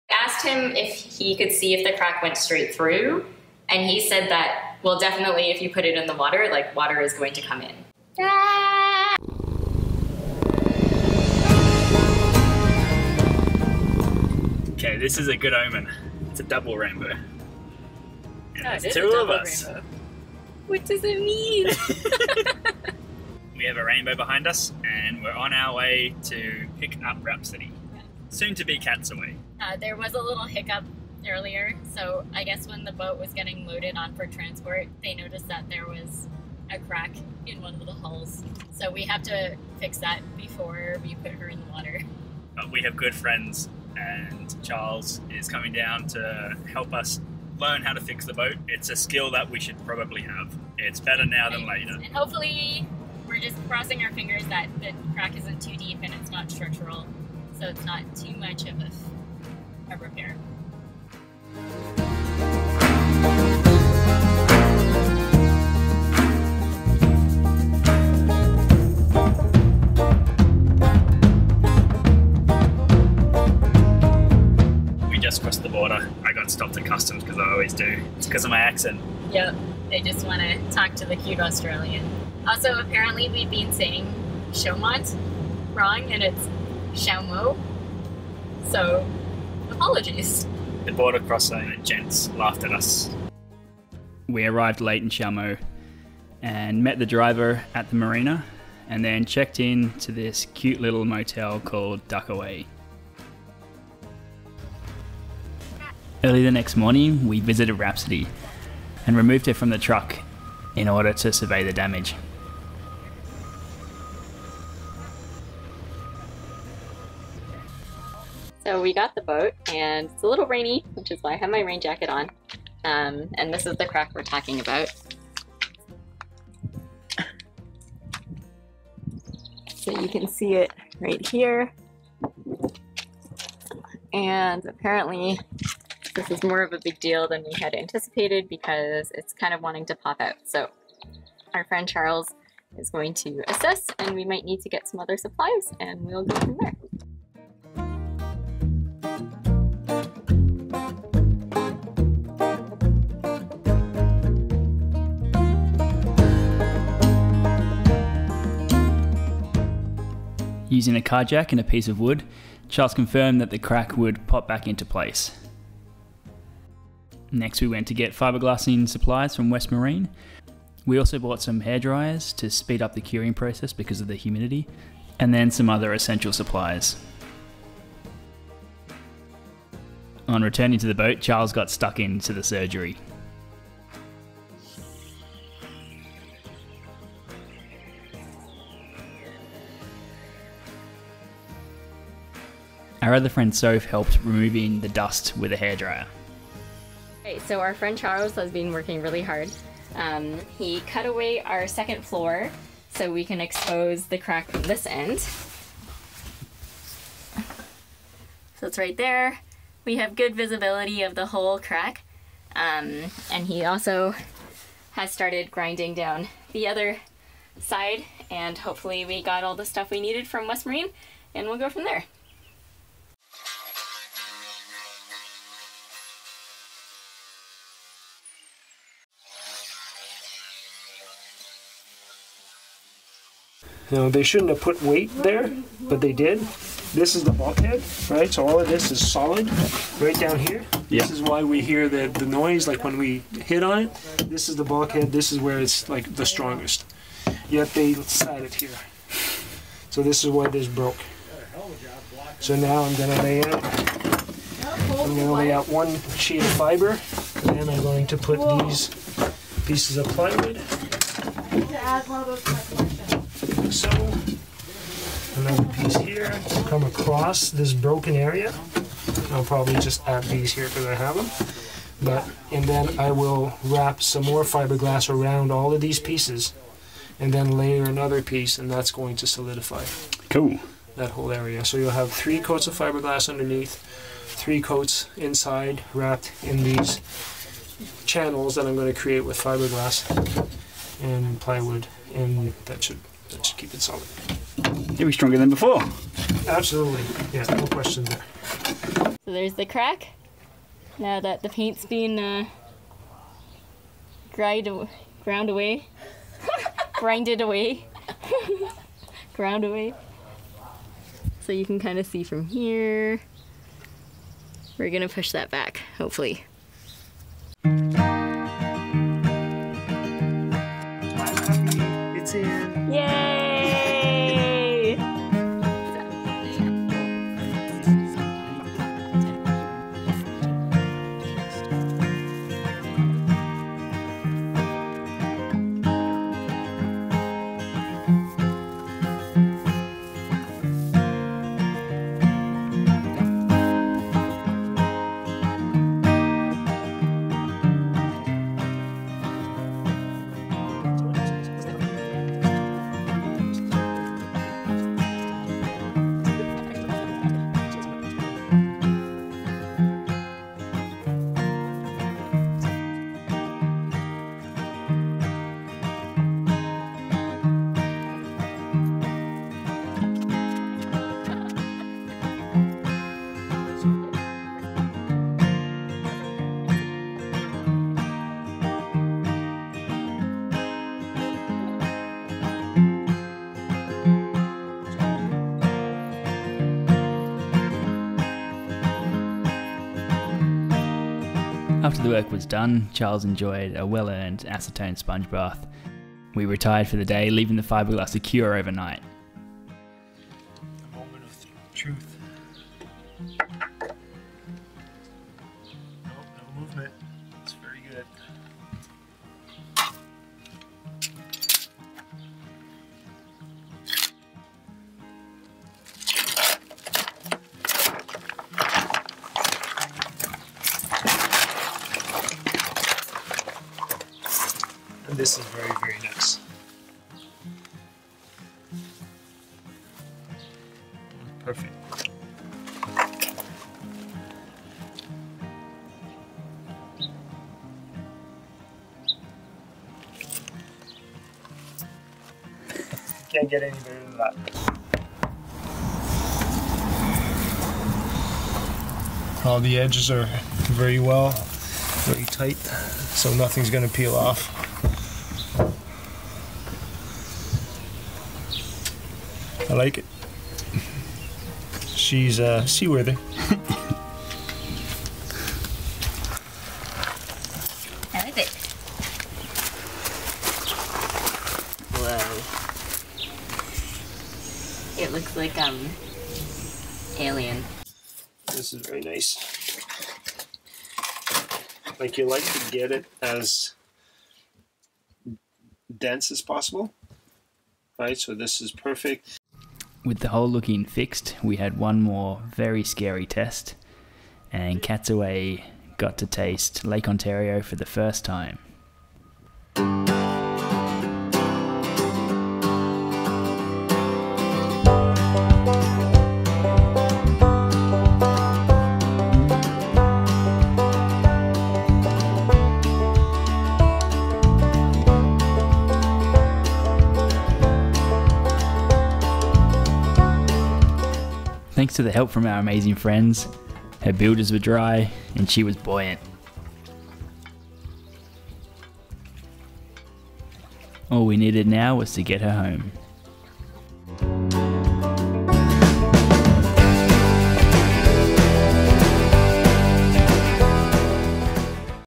Hole in the Hull - we discovered a crack (Sailing SV Catsaway) - Ep. 04
This episode follows the delivery of our boat from Cape Cod, MA to Chaumont, NY.  When the boat was picked up from Cape Cod, a 6 inch crack was discovered in the hull.  By the time it was transported to the Chaumont Yacht Club, the crack had widened to 16 inches.  We repaired the crack with a lot of help and guidance from our friend Charles.  A big thanks also to the Chaumont Yacht Club, for lending us tools and supplies, and for being helpful and encouraging.

After the fiberglass repair was completed, we launched Catsaway into Lake Ontario for her first taste of fresh Canadian water. :)

Thank you Charles, Sophia, Doug, Chris, Geordie and the members and employees of the CYC.

List of supplies/equipment we used:
-West Marine West System 105 Epoxy Resin
-West Marine West System 205 Fast Hardener
-West Marine Fiberglass Mat
-Acetone
-Rollers
-Paintbrush
-Tray for the epoxy
-Measuring Tape
-Mixing Sticks
-Bucket
-Shrink Wrap (but you can use saran wrap too)
-Duct tape
-Grinder
-Hydraulic jack
-Saw
-Plywood
-Hairdryers
-Lots of rags and elbow grease to clean up :)

P.S.  We started to get questions about why the crack wasn't noticed into the survey.  Here are our thoughts:

When we surveyed the boat, we noticed slight recession of the hull, but no crack.  We actually have close up pictures of that exact spot!  Our theory on why the crack happened:
1) the original owner had switched hauling companies this past spring and they asked for a different set up with long wooden beams (he was using cinder blocks and plywood previously).
2) We think the new company set down the boat at the wrong place, which caused the hull recession.
3) As the owner prepared the boat for transport, he added more load (including filling the fuel tanks).

Our best guess is that the combination of being on the hard for a longer length of time than normal (resting in the wrong spot) + slightly more load caused the hull the crack between our survey and transport day.

PS: We really like Josh Woodward